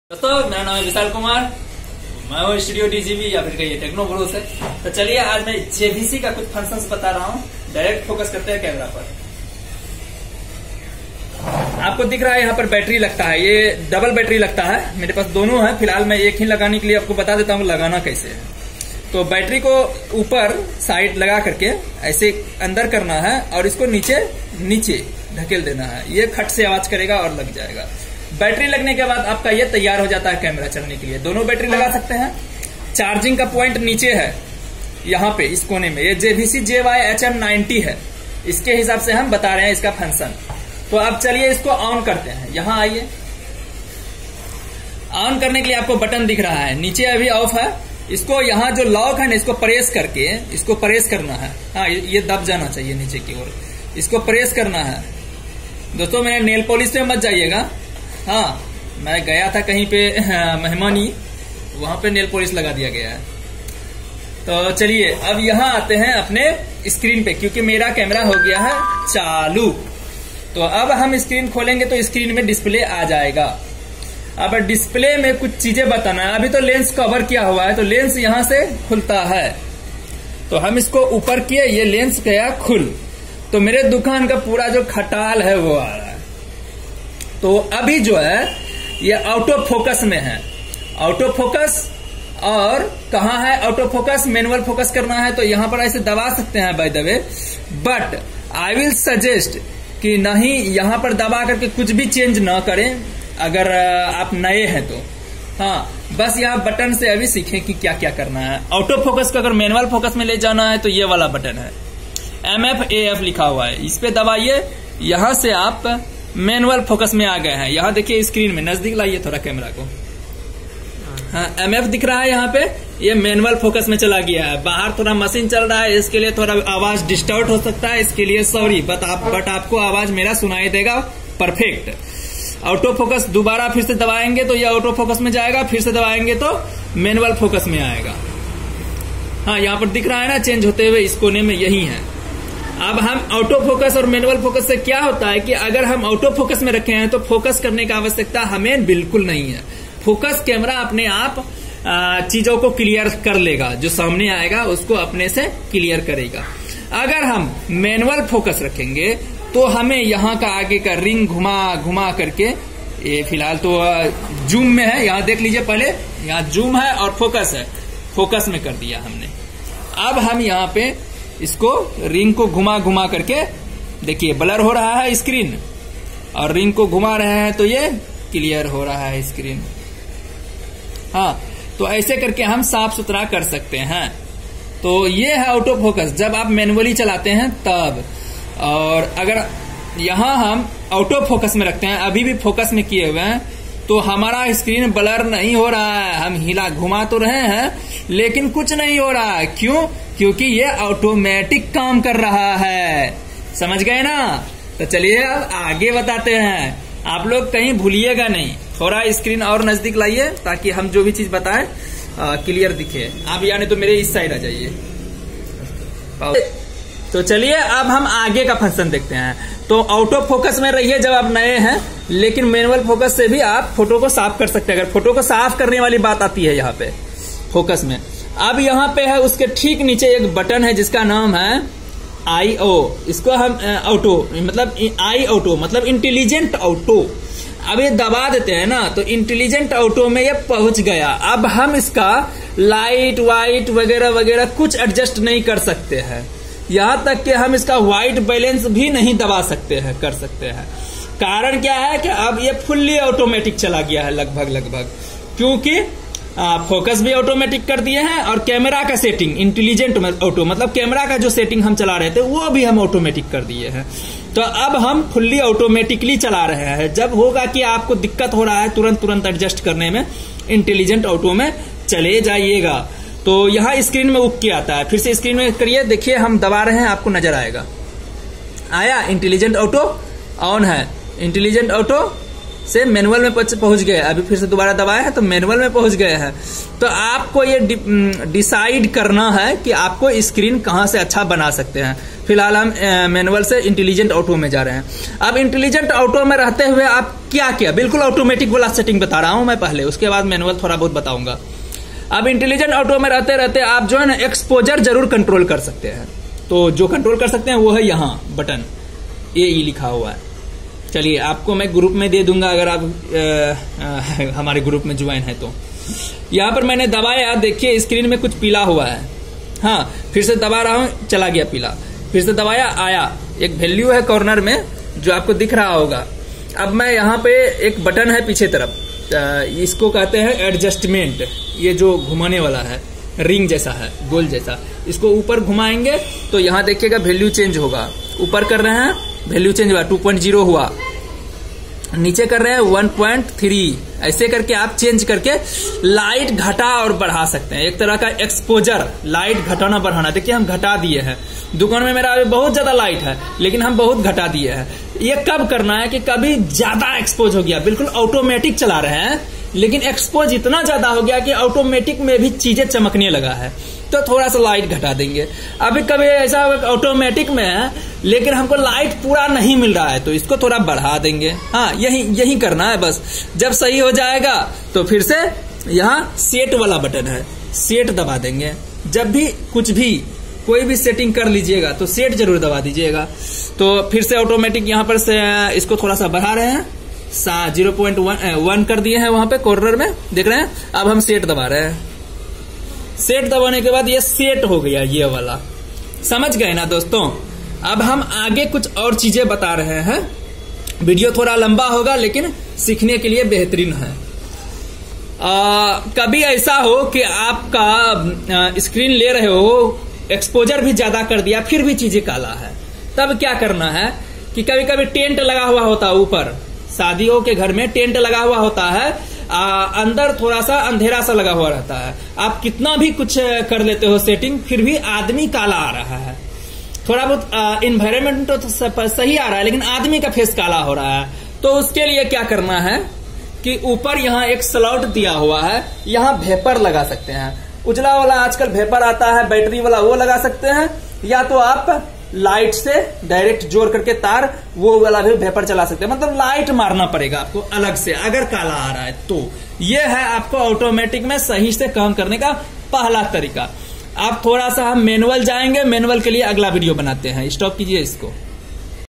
JVC JY HM 90 kaise chalayenge.
दिए गए लिंक से आप मेनुअल सेटिंग के बारे में सिख सकते हैं।

JVC ka ye model ek low budget camera hai. ise professionally bhi use kia ja sakta hai. isme bhi bahut achhe features hai.

Dosto agar aap janana chahte hai ki JVC camera kaise chalaye ya JVC camera ka setting ke bare me janana chahte hai to ye video aapko bahut helpful sabit hoga.

Hame camera ka basic setting jaroor aana chahiye. JVC agar auto me chalayenge to bhi hum AE (Exposure) ko control kar sakte hai.